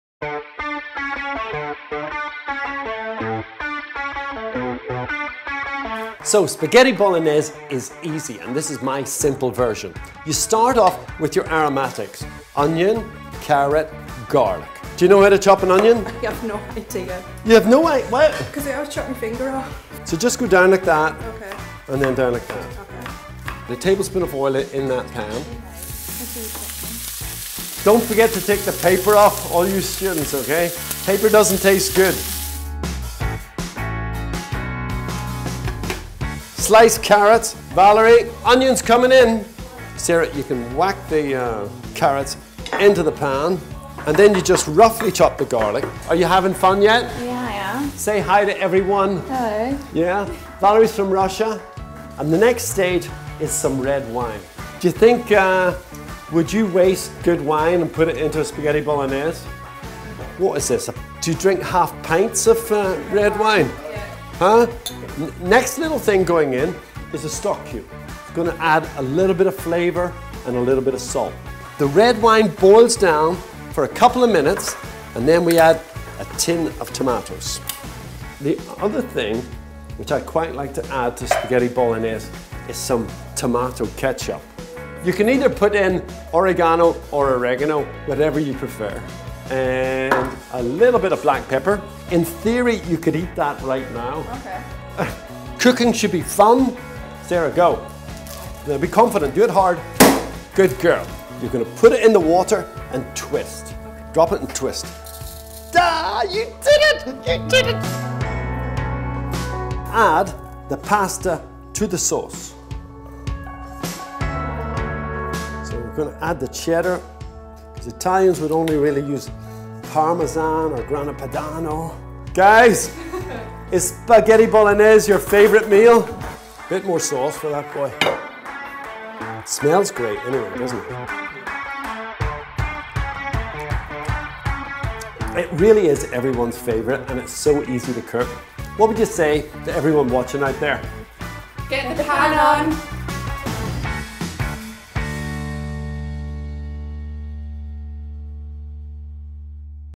Rankin Cooks - Spag Bol
Celebrity Chef Paul Rankin shows us how to make Spag Bol